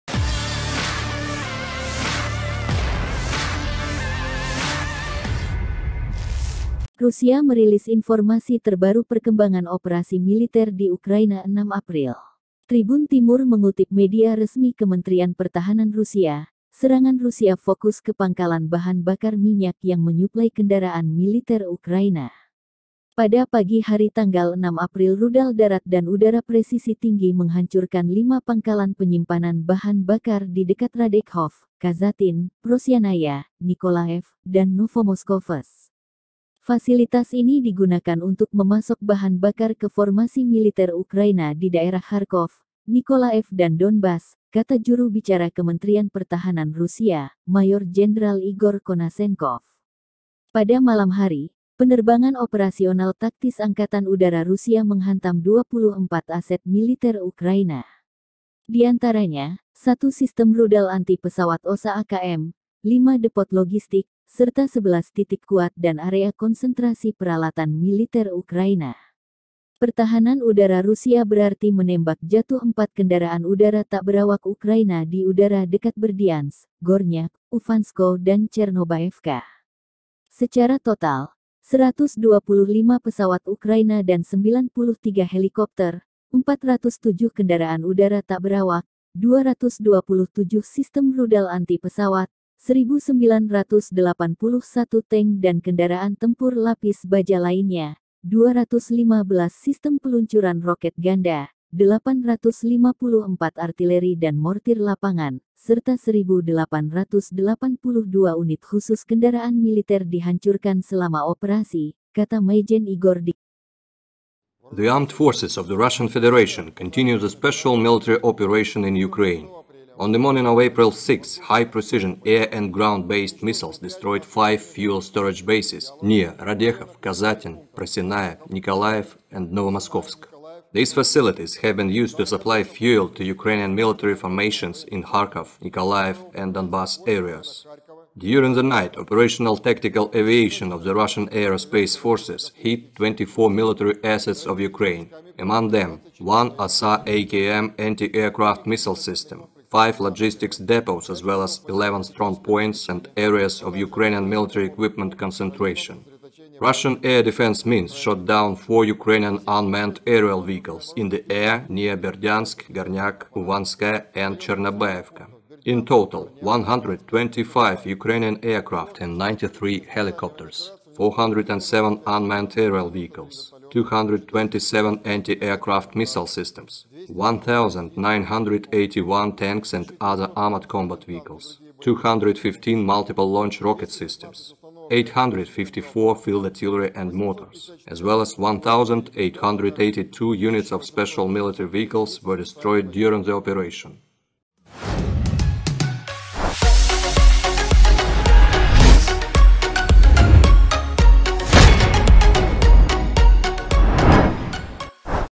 Rudal Bastion Rusia Meluncur dan Berbelok Menuju Ukraina, Pangkalan BBM Hancur Seketika
Video Editor: Mansur AM
Sumber: Kemenhan Rusia

(TRIBUN-TIMUR.COM)